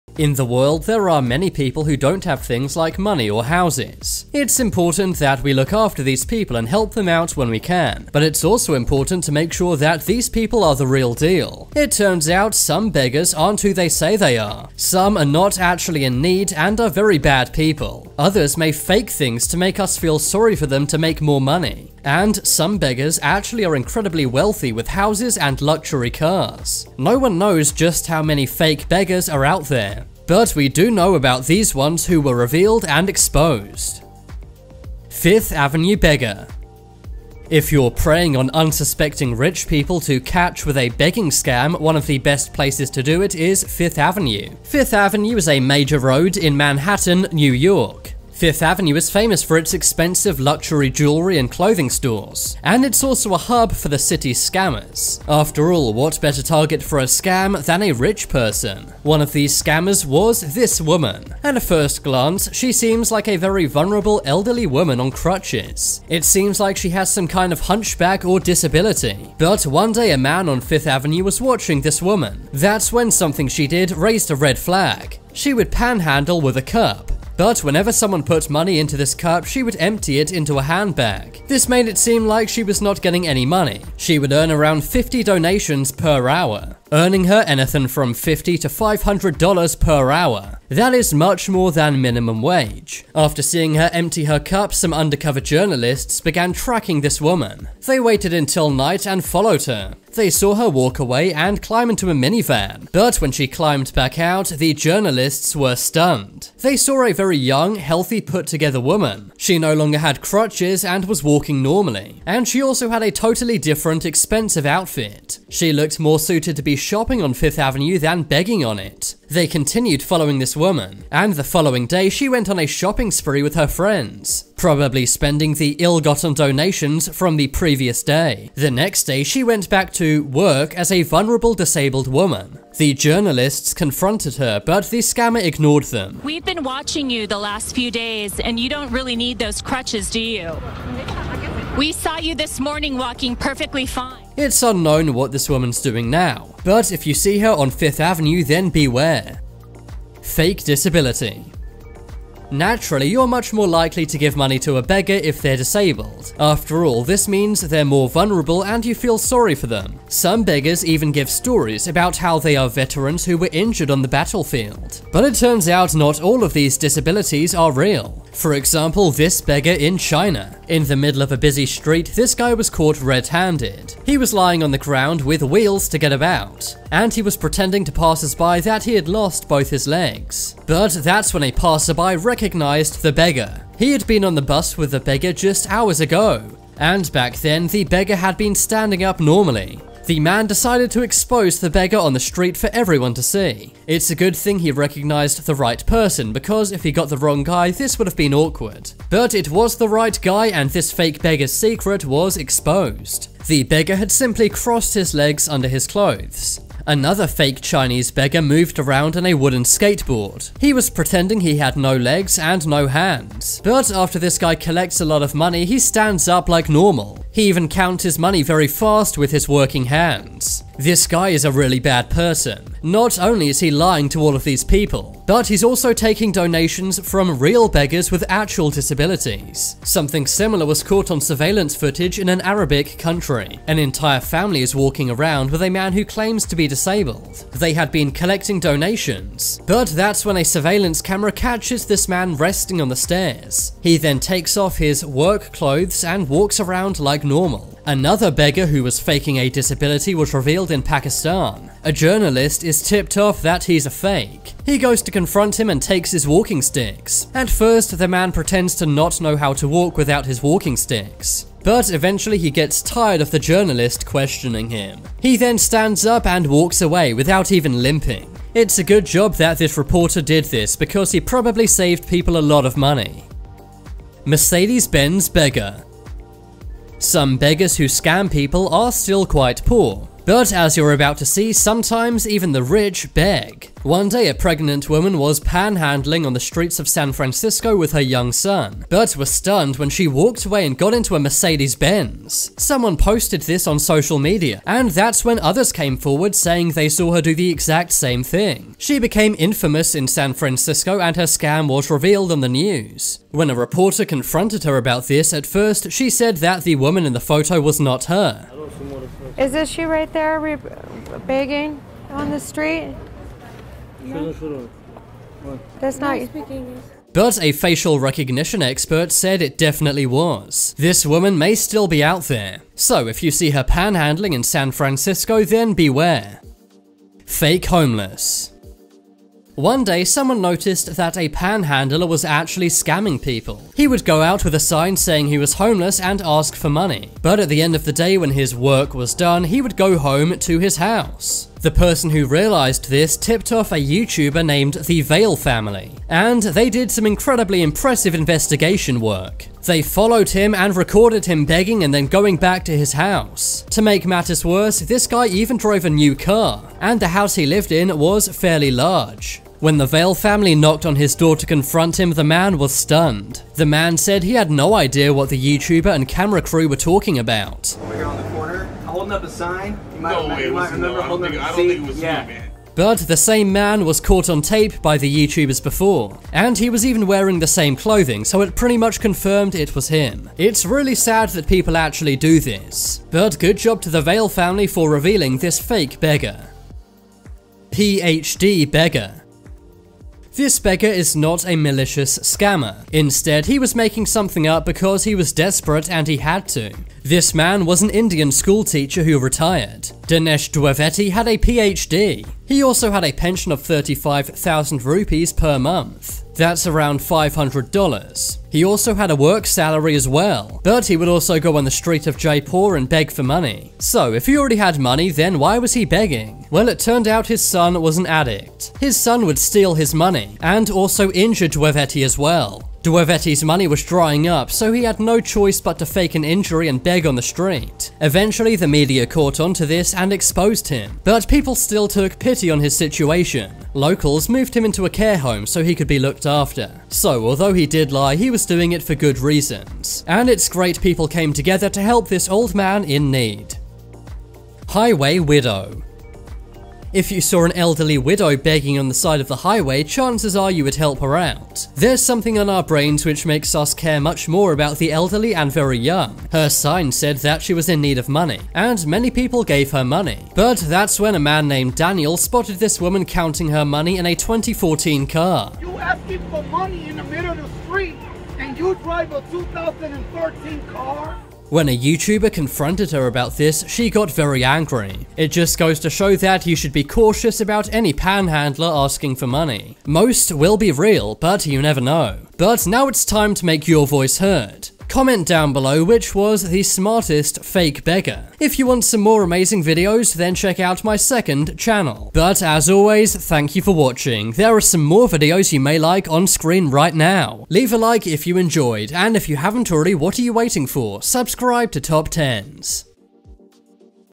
Fake Beggars Who Were Caught Red Handed
Top 10 Fake Beggars Who Were Caught Red Handed

Description:
Charlie from Top 10s counts down the Top10 Fake Beggars Who Were Caught Red Handed! Here are some beggars who were revealed to be fake! From a panhandler who drives a Mercedes-Benz car to a homeless man who has a mansion!